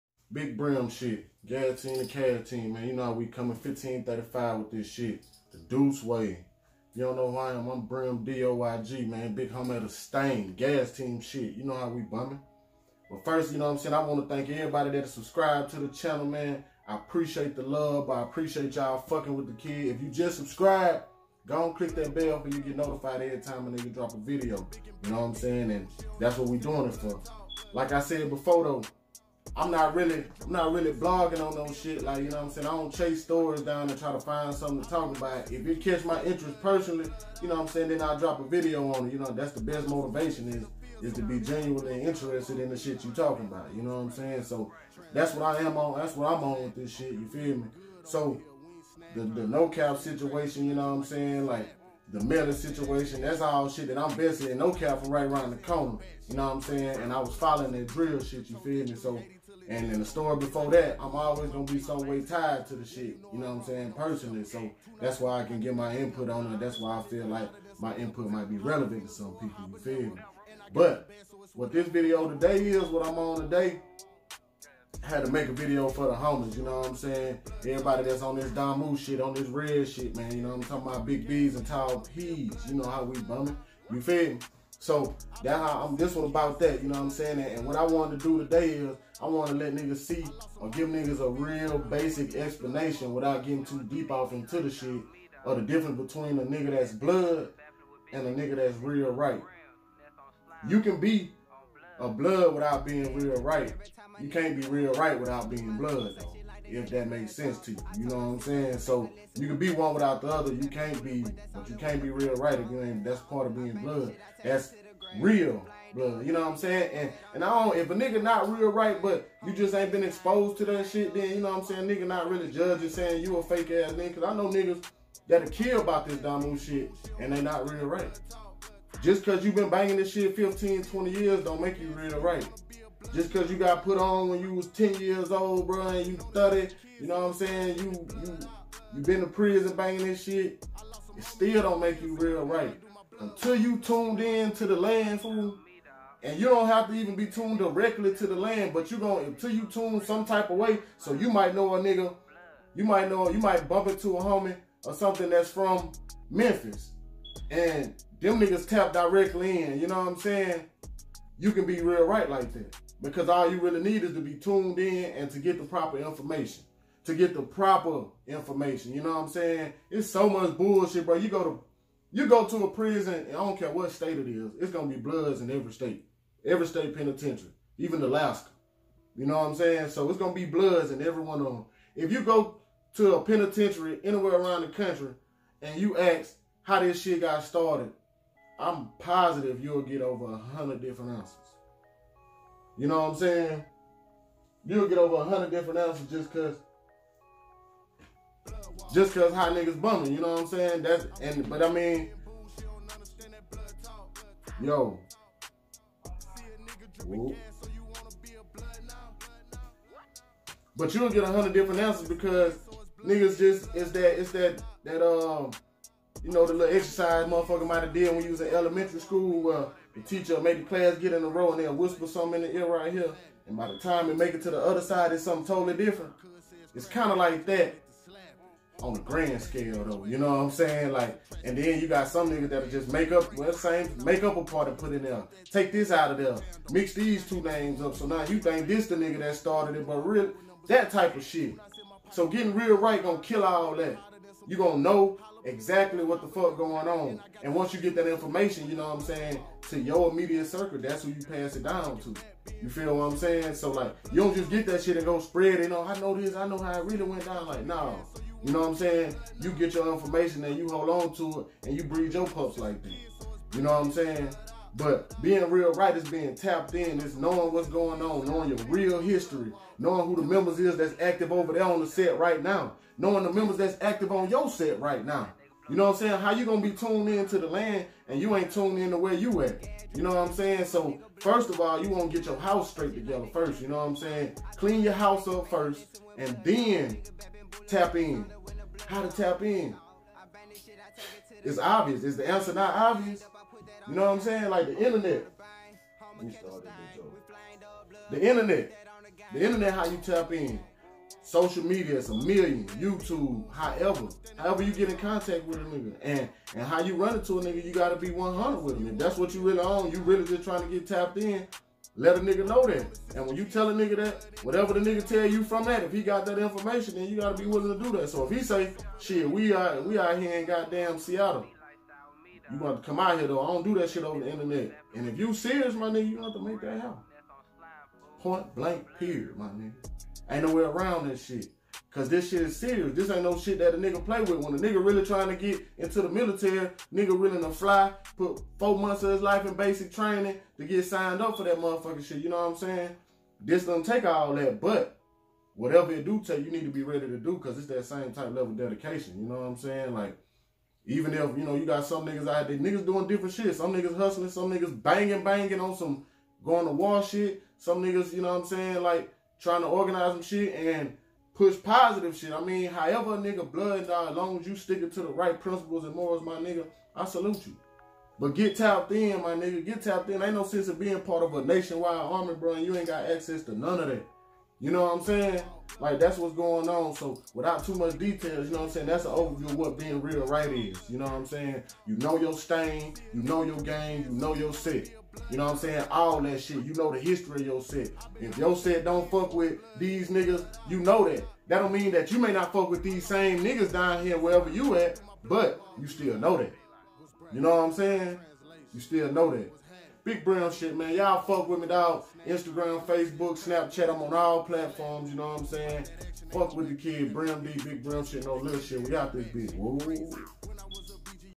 How to spot a REAL Blood | Wat is REAL Right??
On today's showcase Brim D expounds on the difference between a blood and a real right blood member.